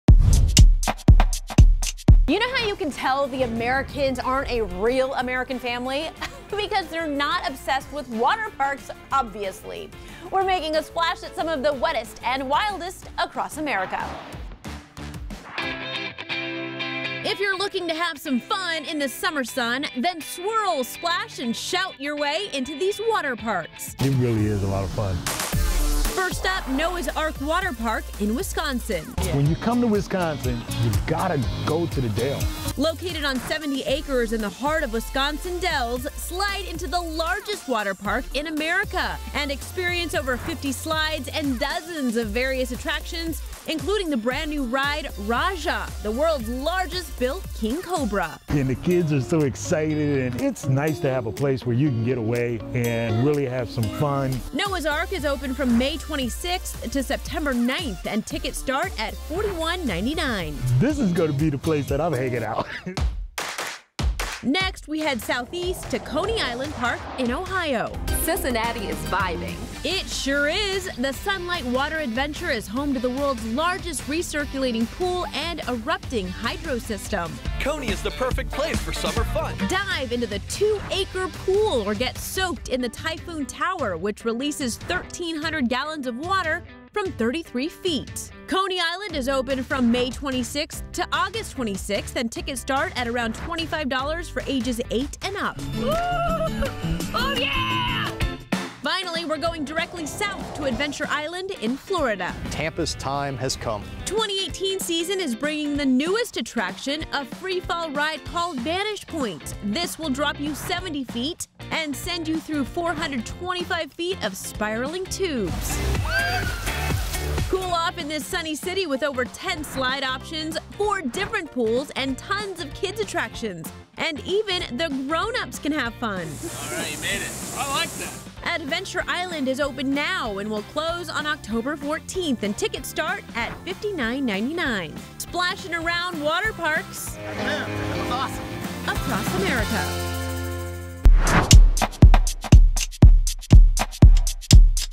Summer Travel Fun: 3 of the Best, Wildest Water Parks Across the U.S.
Summer is coming and nothing beats heading to a water park with your friends and family. We found some of the best water parks, across America.